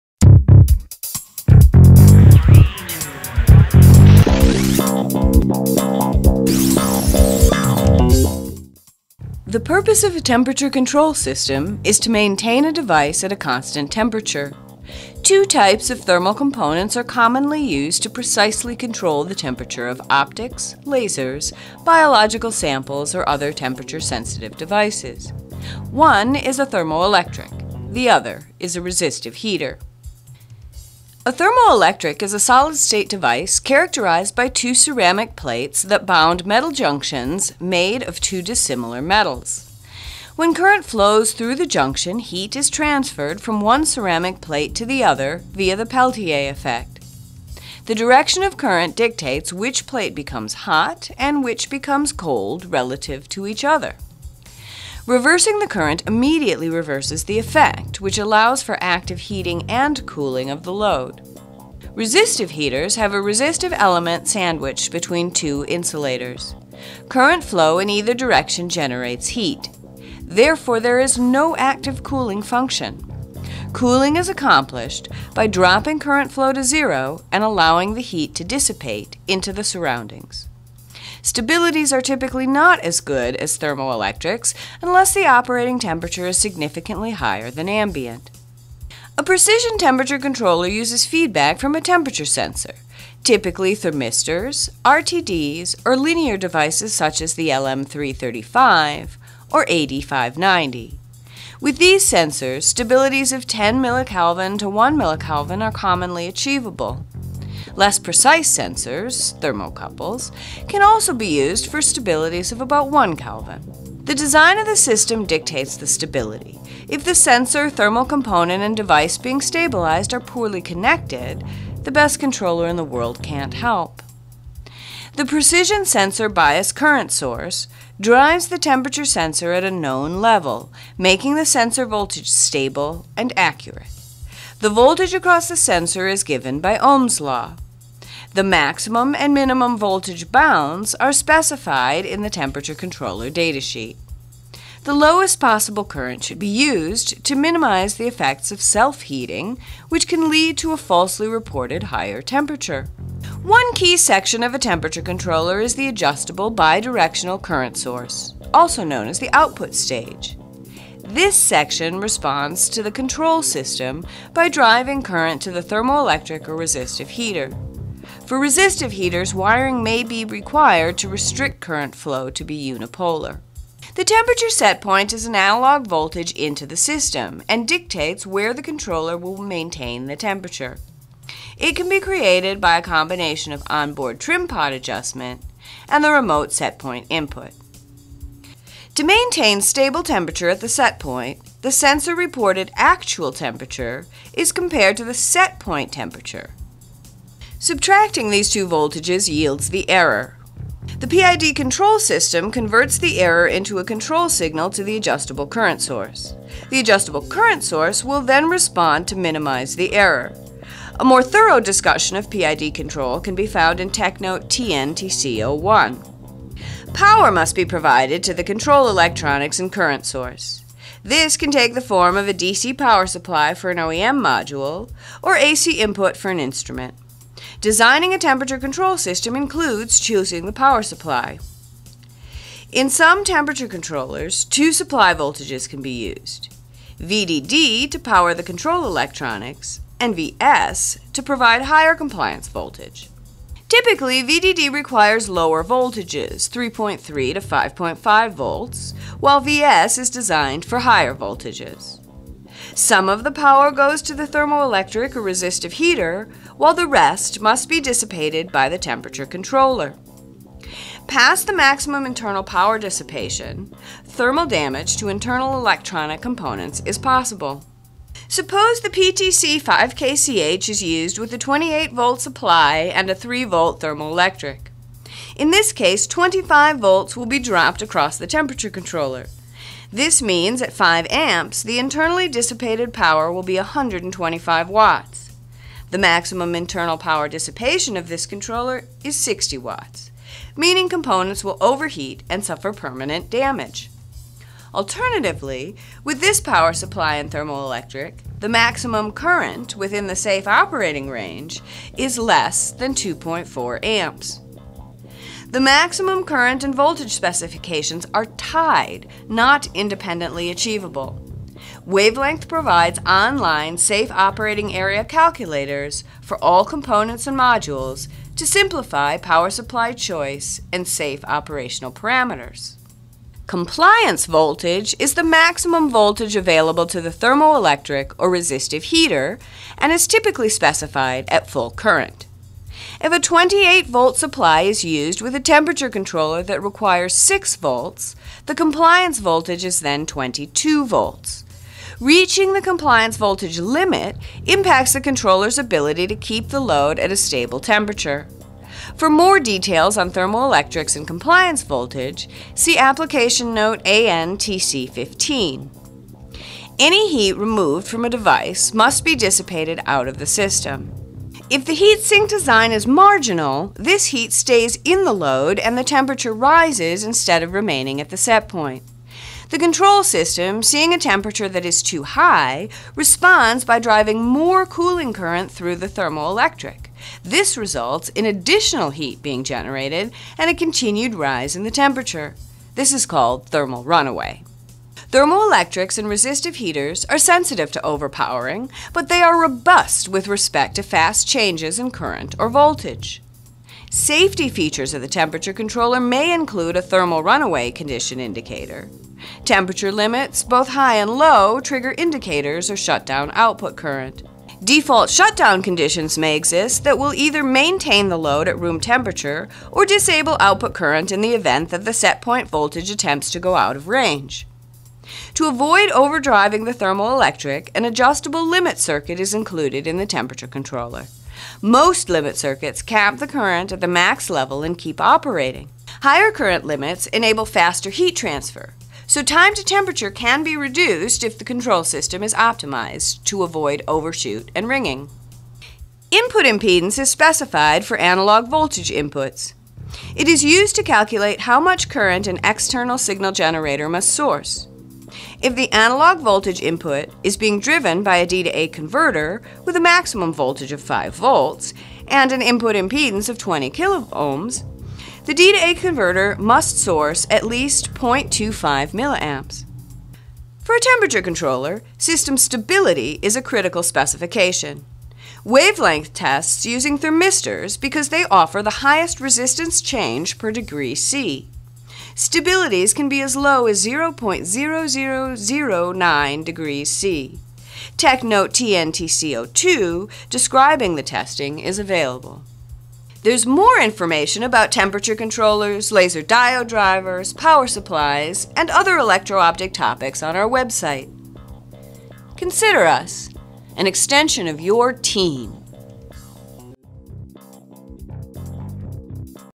What is a Temperature Controller?
The purpose of a temperature control system is to maintain a device at a constant temperature. Two types of thermal components are commonly used to precisely control the temperature of optics, lasers, biological samples, or other temperature sensitive devices. One is a thermoelectric.  The other is a resistive heater.